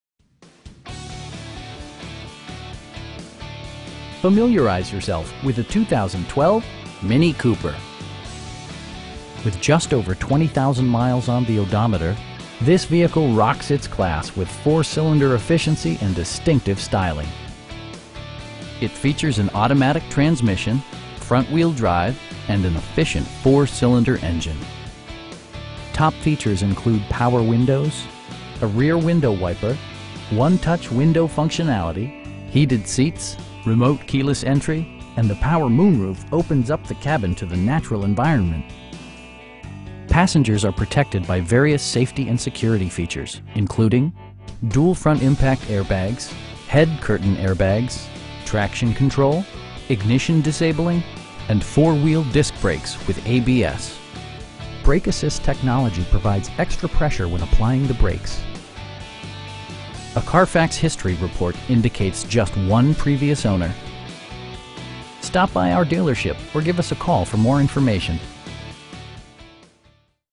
2012 MINI Cooper in Richmond, VA 23294
Richmond BMW
8710 Broad St.

Familiarize yourself with the 2012 MINI Cooper. With just over 20,000 miles on the odometer, this vehicle rocks its class with 4-cylinder efficiency and distinctive styling! It features an automatic transmission, front-wheel drive, and an efficient 4 cylinder engine. Top features include power windows, a rear window wiper, 1-touch window functionality, heated seats, tilt and telescoping steering wheel, remote keyless entry, cruise control, and the power moon roof opens up the cabin to the natural environment. Passengers are protected by various safety and security features, including: dual front impact airbags, head curtain airbags, traction control, ignition disabling, and 4 wheel disc brakes with ABS. Brake assist technology provides extra pressure when applying the brakes. A Carfax history report indicates just one previous owner. We know that you have high expectations, and we enjoy the challenge of meeting and exceeding them! Stop by our dealership or give us a call for more information.